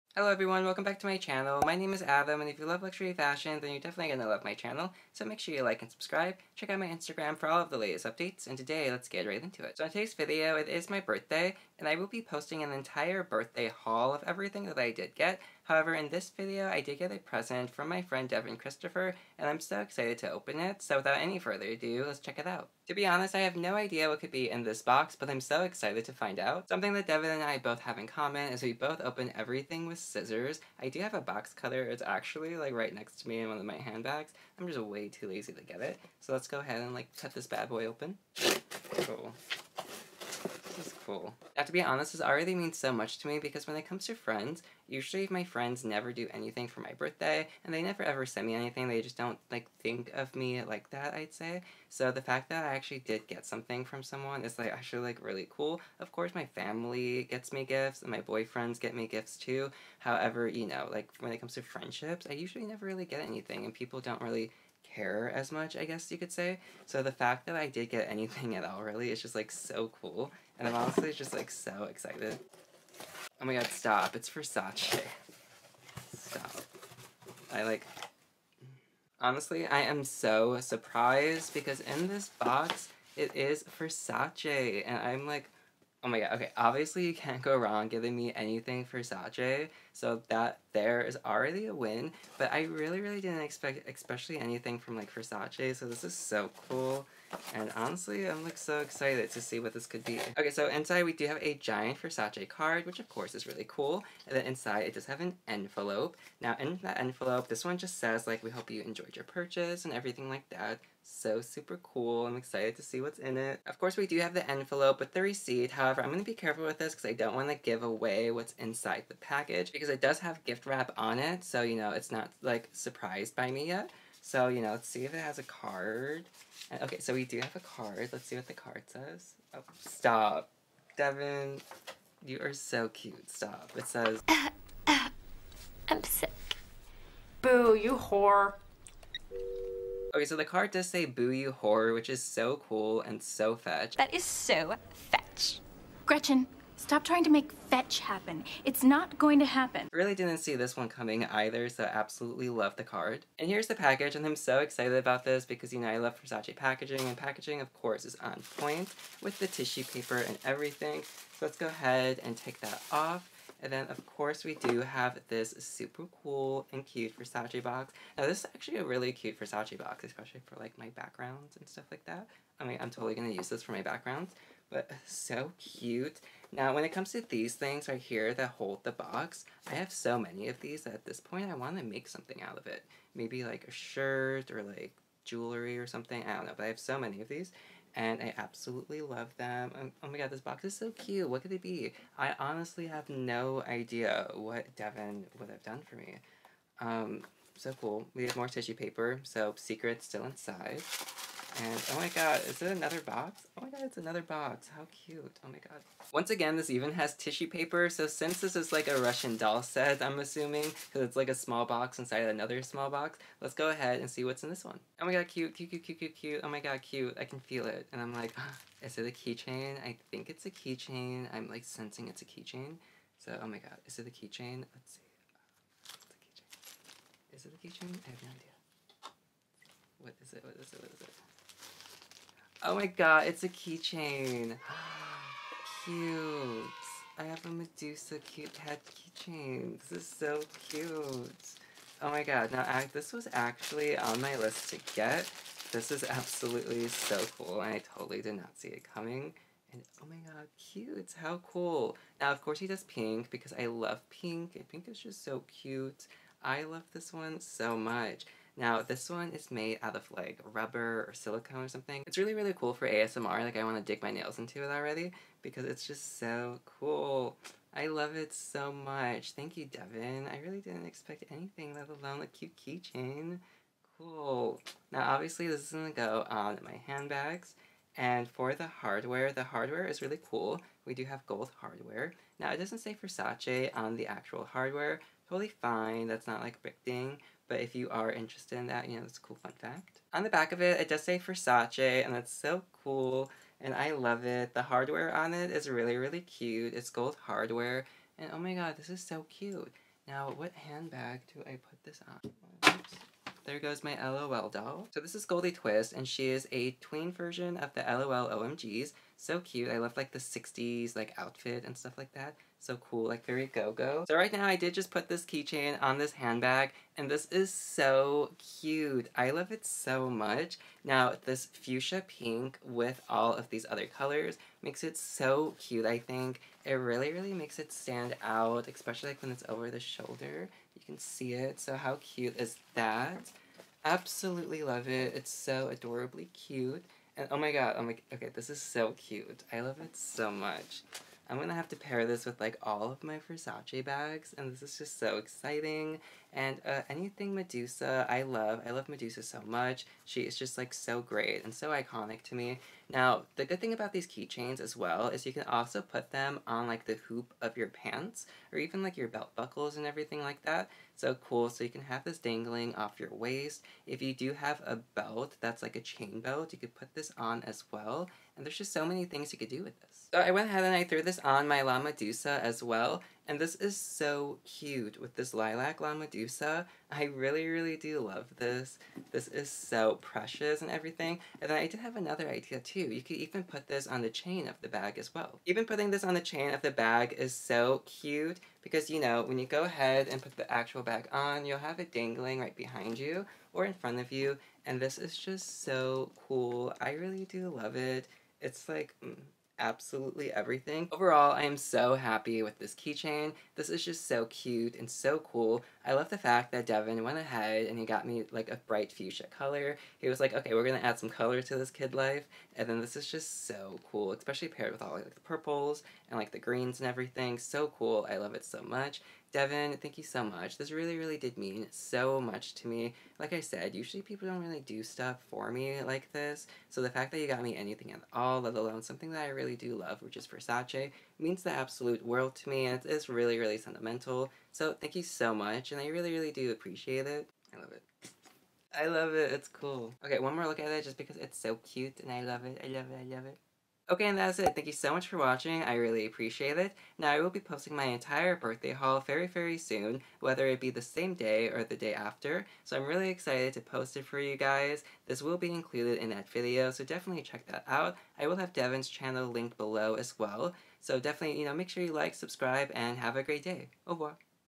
VERSACE MEDUSA KEYCHAIN! (Gift from Devin Christopher!!!)
Hi, I'm Adam!

Today let’s unbox a Birthday Present from my friend Devin Christopher! November 5th was my birthday and he was kind enough to send me a little something, a Versace Keychain! This keychain retails for $125 US dollars and telling from this amazing quality it is 100% worth it! Link will be below!